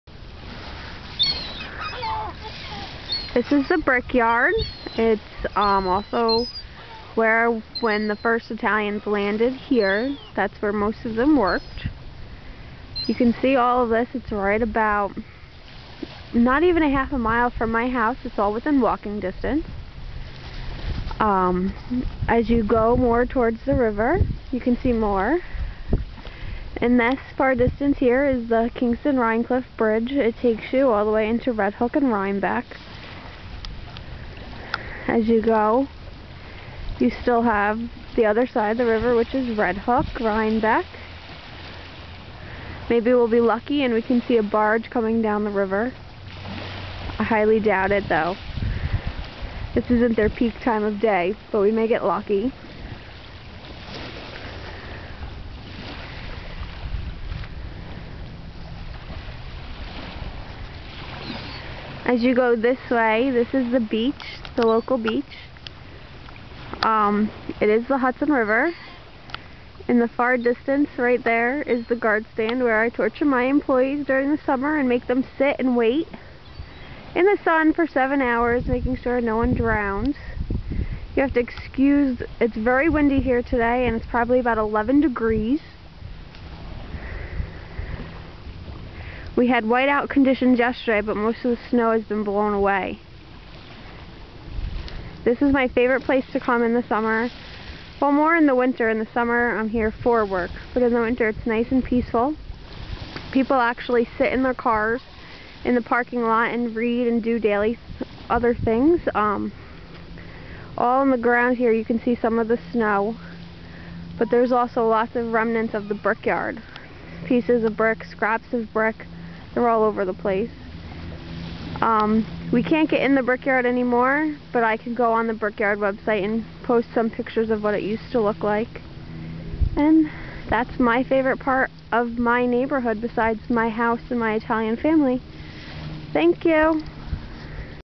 Kingston Point Beach
This is the Hudson River in New York State.  It is less than a half mile from my house.  It is within walking distance from my house.  There is a brick yard where all of the Italian Immigrants worked when they came over from Belsito, Calabria.  The beach is also where I work during the summer and the guard stand is where we suffer on the hot days.  It is a gorgeous place to just come and sit and watch the water.  In the distance there is the Kingston-Rhinecliff Bridge which will take you into Redhook, and that is what is across the river.